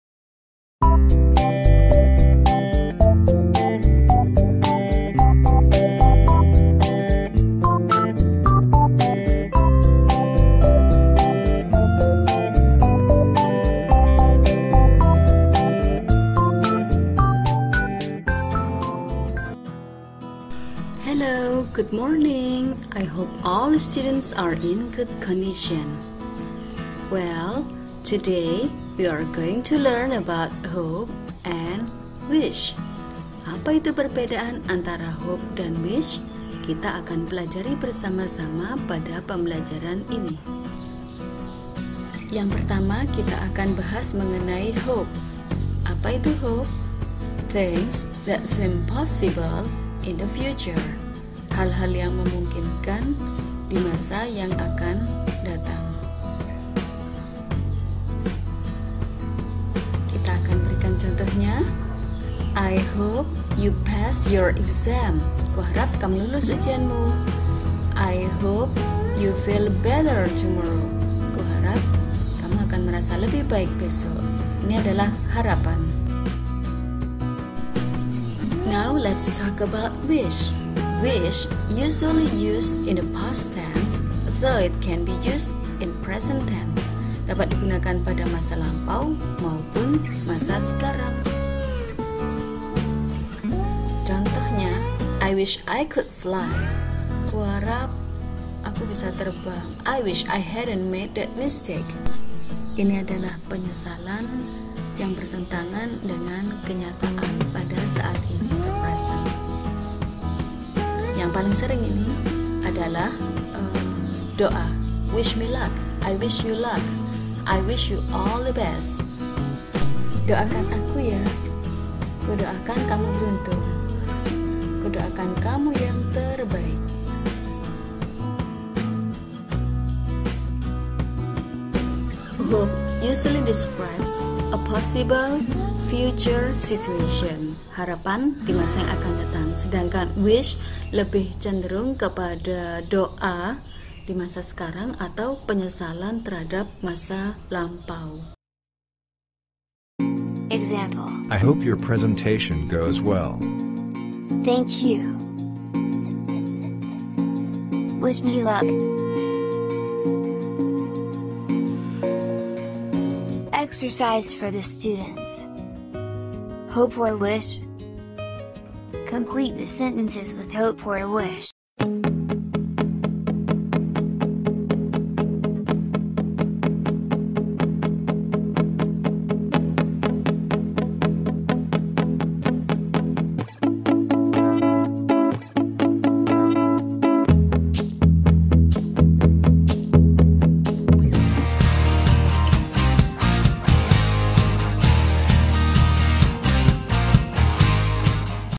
HOPE VS WISH (PERBEDAAN HOPE WISH BAHASA INGGRIS)
Pertanyaan yang sering ditanyakan murid-murid, "Apa bedanya hope dengan wish?" secara arti hope dan wish sama-sama berharap.
Pada video ini, HOPE VS WISH (PERBEDAAN HOPE WISH BAHASA INGGRIS) kita akan belajar mengenai perbedaan antara Hope dengan Wish, penerapannya dalam kalimat dan situasi yang berbeda.

 Petunjuk Belajar HOPE VS WISH (PERBEDAAN HOPE WISH BAHASA INGGRIS):
1. Amati penjelasannya
2. Lihat contoh kalimatnya
3. Perhatikan contoh percakapannya
4. Kerjakan latihannya, 8 nomor melengkapi kalimat dengan "hope" atau "wish"
selamat belajar

VIDEO PEMBELAJARAN BIG SMP KELAS 9,8,7 GANJIL

LESSON 2
*LET'S LIVE A HEALTHY LIFE*

finding hope wish u felt the way i do,